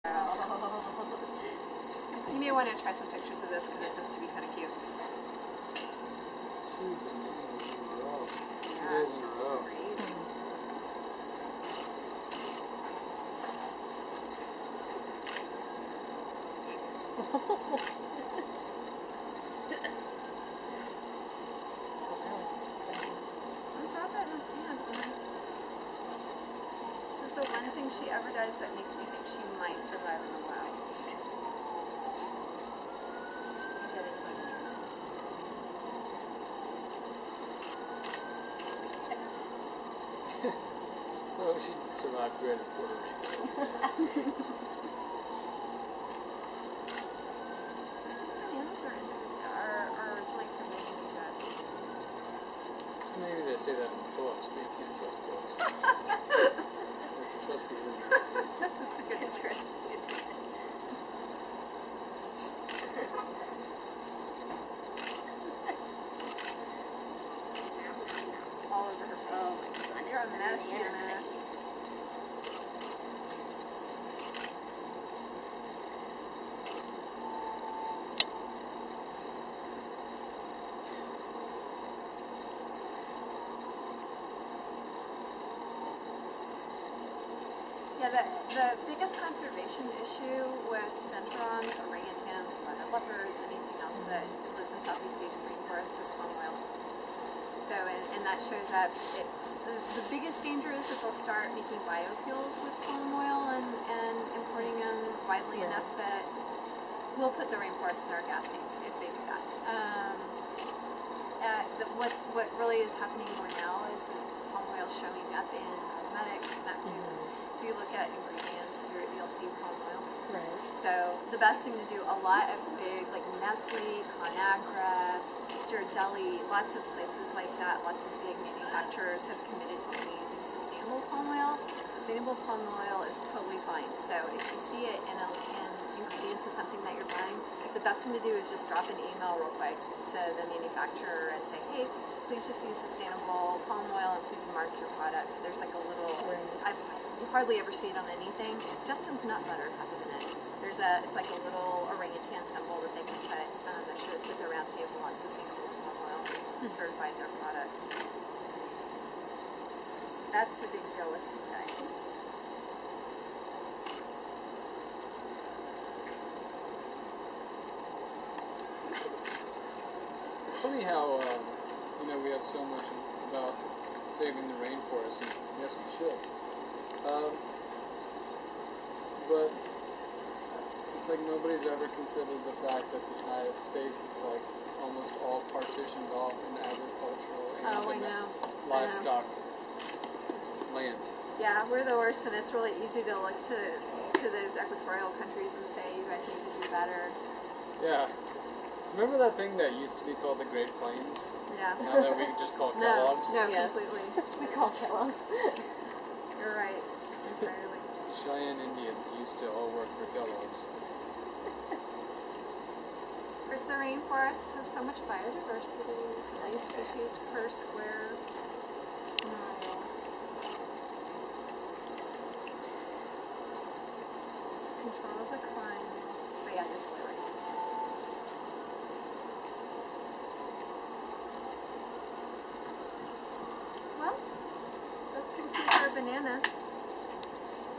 Bearkat Hannah eats a Banana
Little video at the Houston Zoo of their Bearkat enjoying a full banana after a training session.